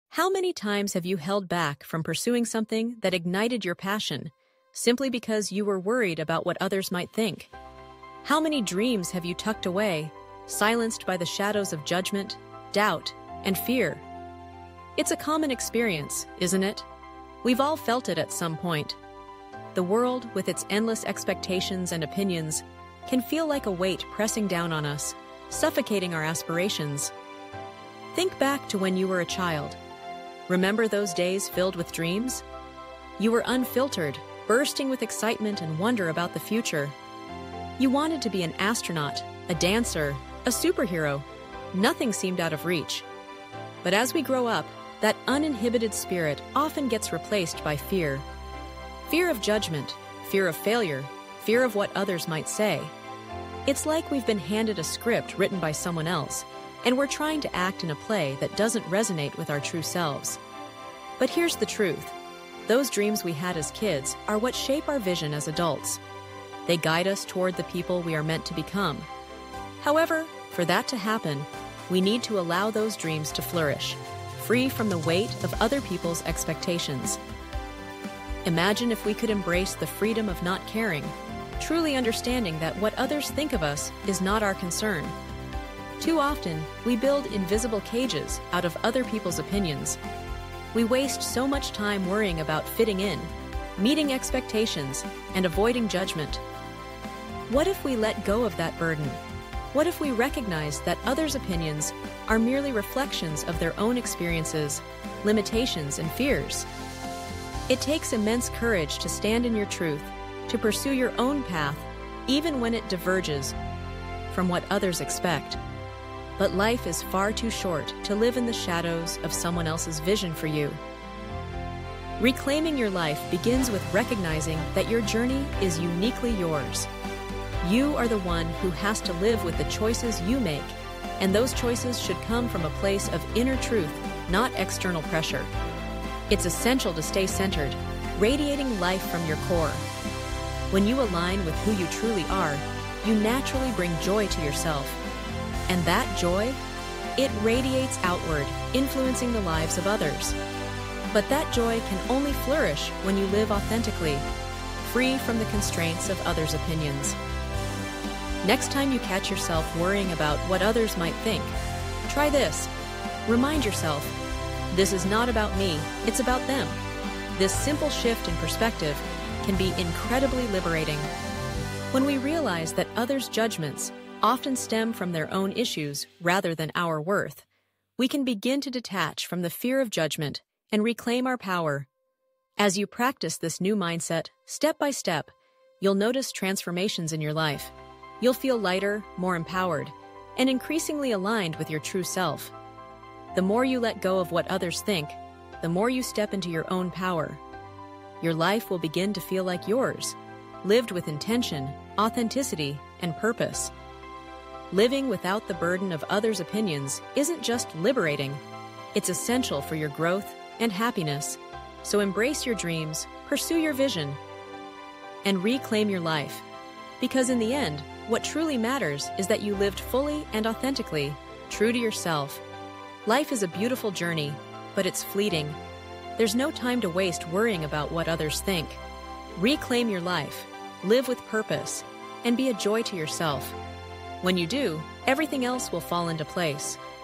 Reclaim Your Dreams, Reclaim Your Life: Stop Worrying About What Others Think. @Motivational speech
Reclaim Your Life: Stop Worrying About What Others Think
How many times have you stopped doing something that your heart truly aspired to do because of what others might think? How many dreams have been quietly tucked away, lost in the shadows of judgment, doubt, and fear?

We all have experienced this at some point. The world, with all its expectations and opinions, can sometimes feel like a heavy burden. We start with dreams as children, uninhibited by the opinions of others, full of wonder and excitement for what the future might hold. But as we grow older, something happens. Those dreams often get replaced by fear—fear of judgment, fear of failure, fear of what others might say.

Yet, it is these dreams that shape our vision, that guide us toward becoming the people we are meant to be. Children with dreams grow up to be adults with vision. But only if we allow those dreams to thrive, free from the weight of others’ expectations.

Embrace the Freedom of Not Caring
What if we embraced the freedom of not caring? What if we truly understood that what others think of us is not our concern?
How many times have you held back from pursuing something that ignited your passion, simply because you were worried about what others might think? How many dreams have you tucked away, silenced by the shadows of judgment, doubt, and fear? It’s a common experience, isn’t it? We’ve all felt it at some point. The world, with its endless expectations and opinions, can feel like a weight pressing down on us, suffocating our aspirations.

Think back to when you were a child. Remember those days filled with dreams? You were unfiltered, bursting with excitement and wonder about the future. You wanted to be an astronaut, a dancer, a superhero—nothing seemed out of reach. But as we grow up, that uninhibited spirit often gets replaced by fear. Fear of judgment, fear of failure, fear of what others might say. It’s like we’ve been handed a script written by someone else, and we’re trying to act in a play that doesn’t resonate with our true selves.

But here’s the truth: those dreams we had as kids are what shape our vision as adults. They guide us toward the people we are meant to become. However, for that to happen, we need to allow those dreams to flourish, free from the weight of other people’s expectations. Imagine if we could embrace the freedom of not caring—truly understanding that what others think of us is not our concern. Too often, we build invisible cages out of other people’s opinions. We waste so much time worrying about fitting in, meeting expectations, and avoiding judgment.

What if we let go of that burden? What if we recognized that others’ opinions are merely reflections of their own experiences, limitations, and fears? It takes immense courage to stand in your truth, to pursue your own path even when it diverges from what others expect. But life is far too short to live in the shadows of someone else’s vision for you.

Reclaiming your life begins with recognizing that your journey is uniquely yours. You are the one who has to live with the choices you make, and those choices should come from a place of inner truth, not external pressure. It’s essential to stay centered, radiating life from your core. When you align with who you truly are, you naturally bring joy to yourself. And that joy? It radiates outward, influencing the lives of others. But that joy can only flourish when you live authentically, free from the constraints of others’ opinions.

Next time you catch yourself worrying about what others might think, try this: remind yourself, “This is not about me. It’s about them.” This simple shift in perspective can be incredibly liberating. When we realize that others’ judgments often stem from their own issues rather than our worth, we can begin to detach from the fear of judgment and reclaim our power.

As you practice this new mindset, step by step, you’ll notice transformations in your life. You’ll feel lighter, more empowered, and increasingly aligned with your true self. The more you let go of what others think, the more you step into your own power. Your life will begin to feel like yours—lived with intention, authenticity, and purpose. Living without the burden of others’ opinions is not just freeing—it’s essential for your growth and happiness. So, embrace your dreams, pursue your vision, and reclaim your life. Because in the end, what truly matters is that you lived fully and authentically, true to yourself.

Conclusion
Life is a beautiful journey, but it is also fleeting. There is no time to waste worrying about what others think. Reclaim your life, live with purpose, and be a joy to yourself. When you do, you’ll find that everything else falls into place.

PersonalGrowth
SelfImprovement
ReclaimYourLife
StopCaringWhatOthersThink
AuthenticLiving
CourageToBeYourself
Mindfulness
InnerPeace
LiveYourTruth
PositiveMindset
EmbraceChange
MentalWellbeing
VisionAndDreams
Empowerment